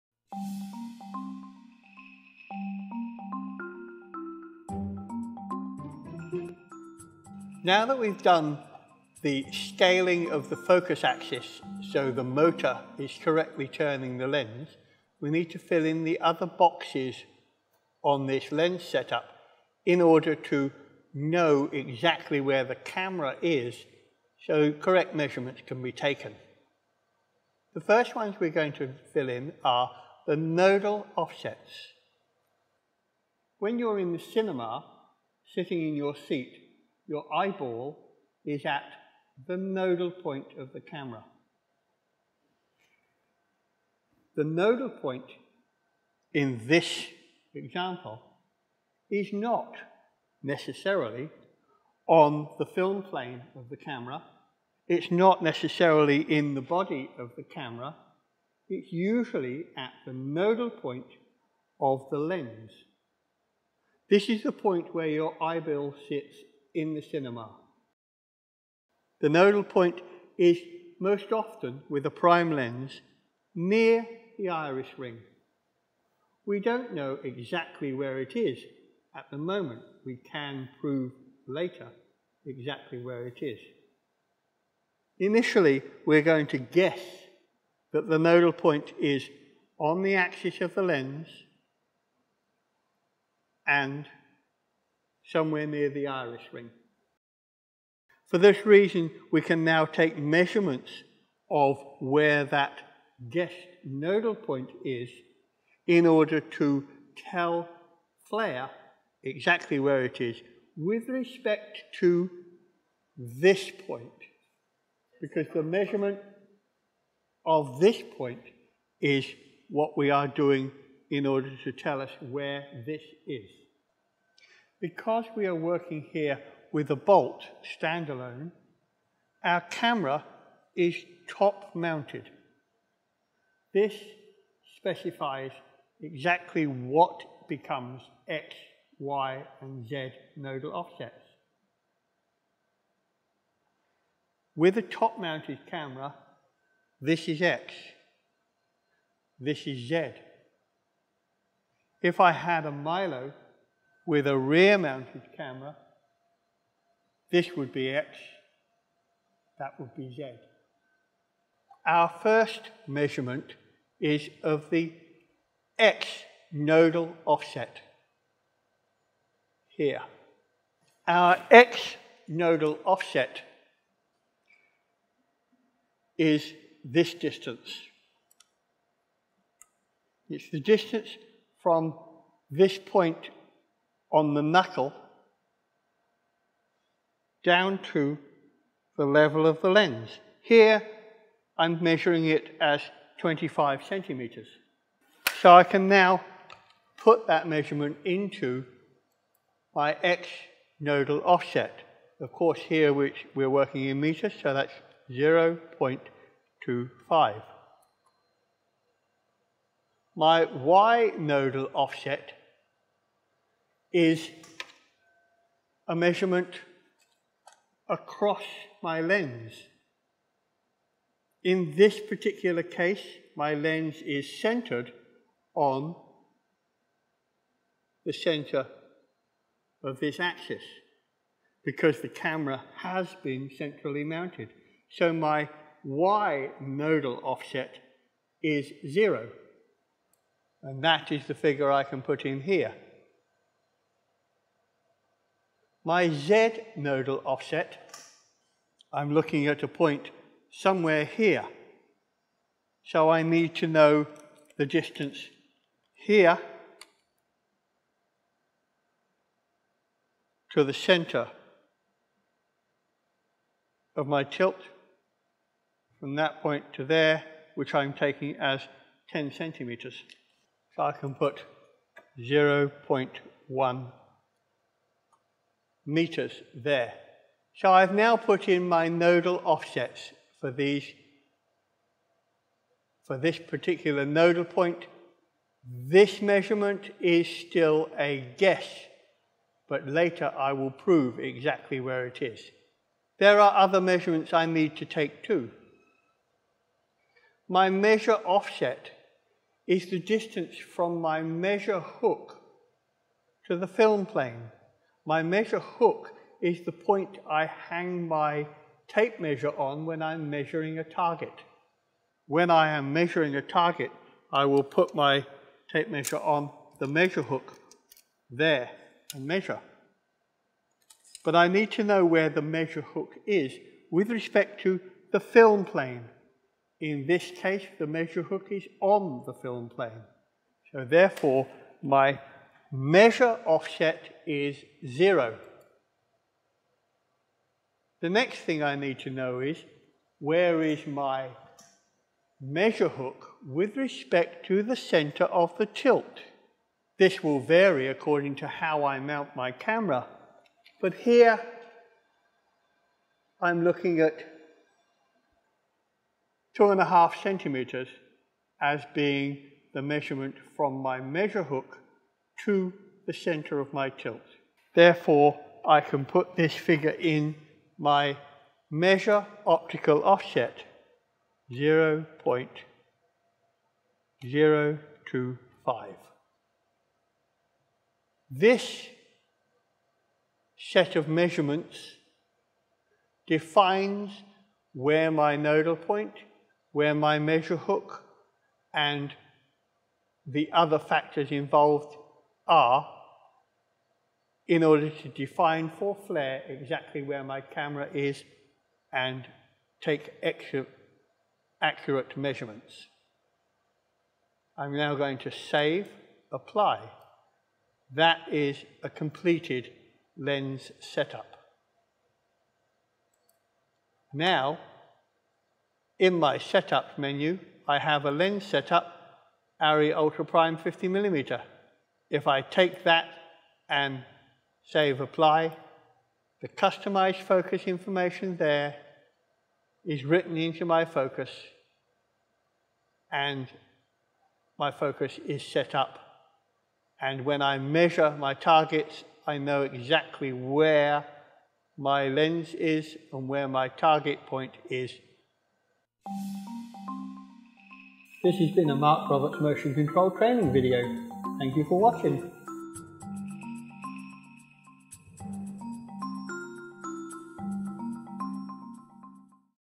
07 - Lens Calibration Part 2
Understand the relation between the lens and the robot, and learn how to measure the nodal point of the lens.